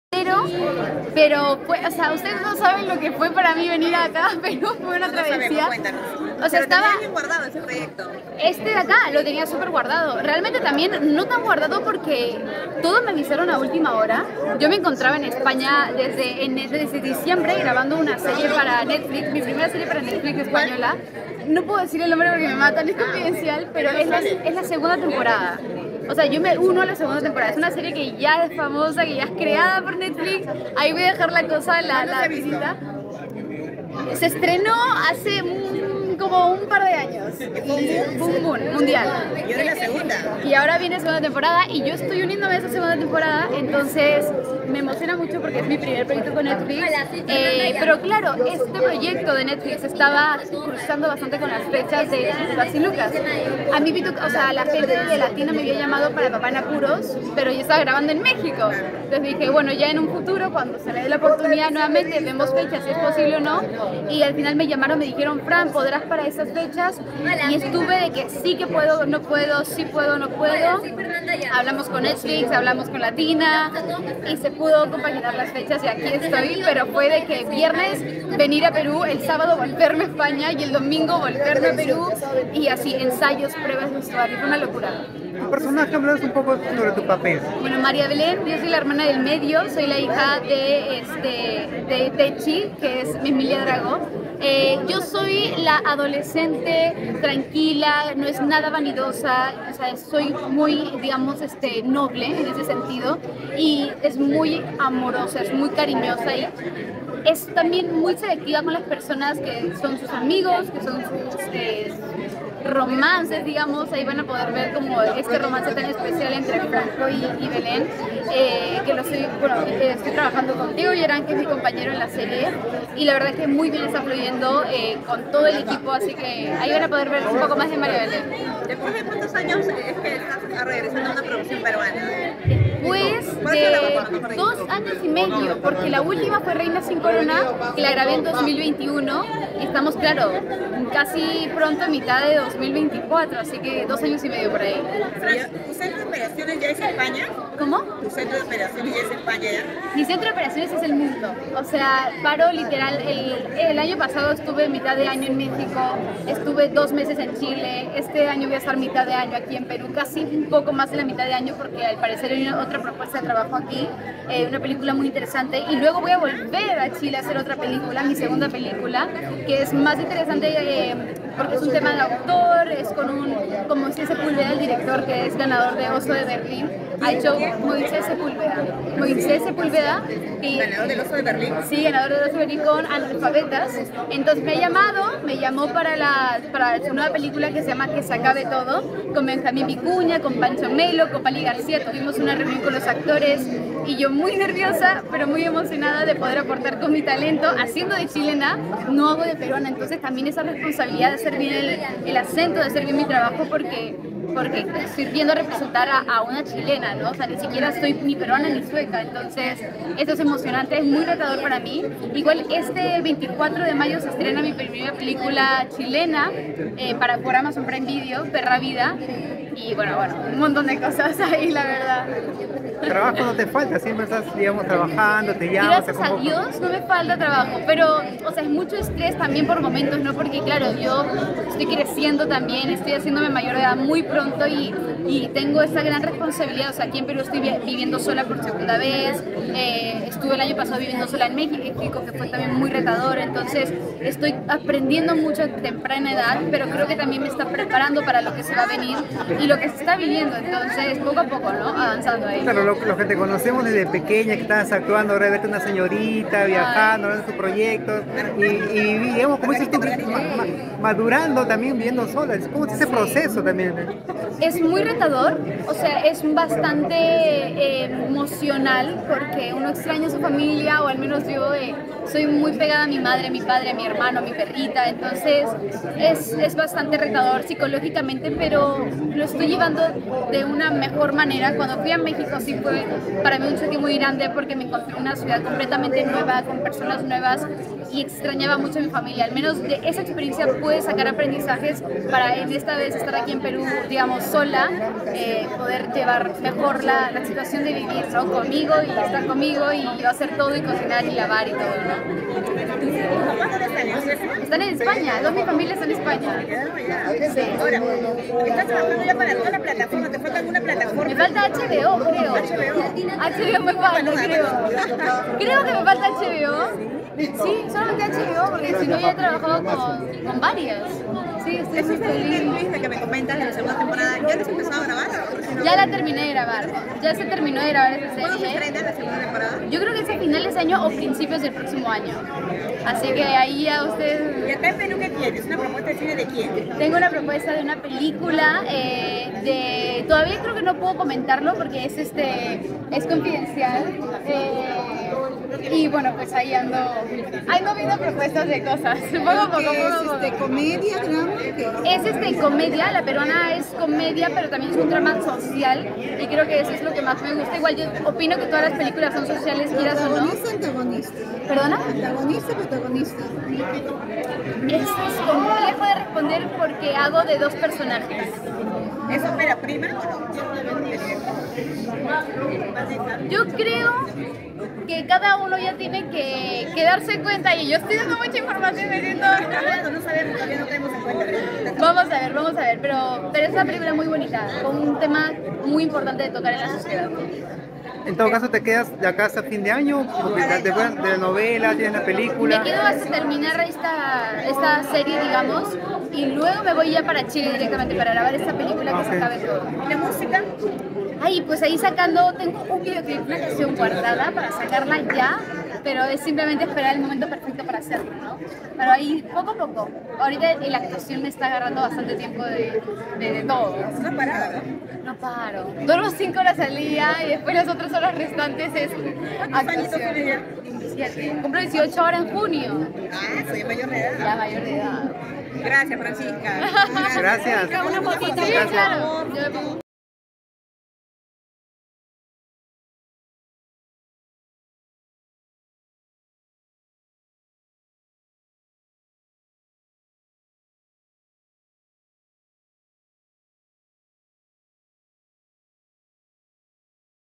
FRANCISCA ARONSSON: ACTRIZ PERUANA MÁS INTERNACIONAL DEL MOMENTO
La joven actriz nacional FRANCISCA ARONSSON, que forma parte de la telenovela "Pituca sin Lucas" que emite Latina, habla del buen momento laboral y de la serie que grabó para Netflix. Producción que en su primera temporada fue un boom a nivel mundial y donde ella participa en la secuela. Recuerda sus inicios en la actuación y de como su profesión la llevó a trabajar a diversos países del mundo.